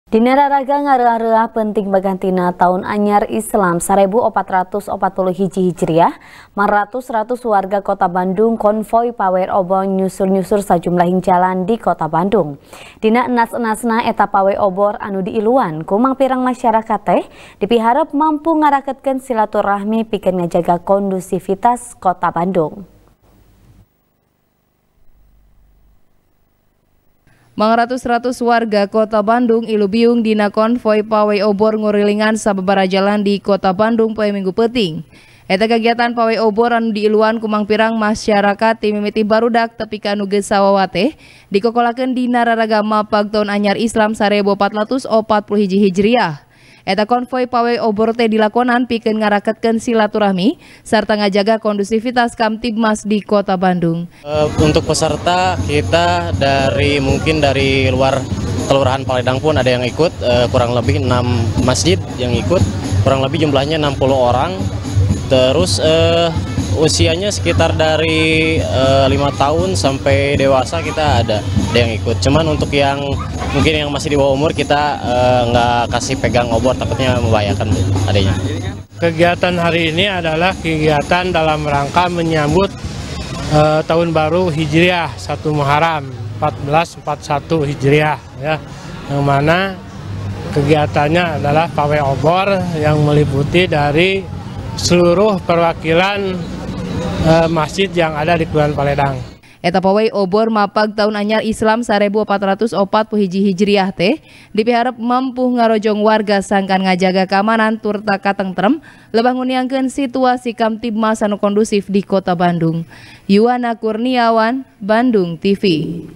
Kariaan Pawey Obor Mieling Taun Anyar Islam 1441 Hijriah | TP | BANDUNGTV
TP | BANDUNGTV | ASTRI CAHYANI | Dina Raraga Ngareuah-Reuah Peuting Bagantina Taun Anyar Islam 1441 Hijriah, Mangratus-Ratus Warga Kota Bandung Konvoi Pawey Obor Nyusur-Nyusur Sajumlahing Jalan Di Kota Bandung. Dina Enas-Enasna, Eta Pawey Obor Anu Diiluan Ku Mangpirang Masarakat Teh, Dipiharep Mampuh Ngaraketkeun Silaturahmi, Pikeun Ngajaga Kondusifitas Kota Bandung.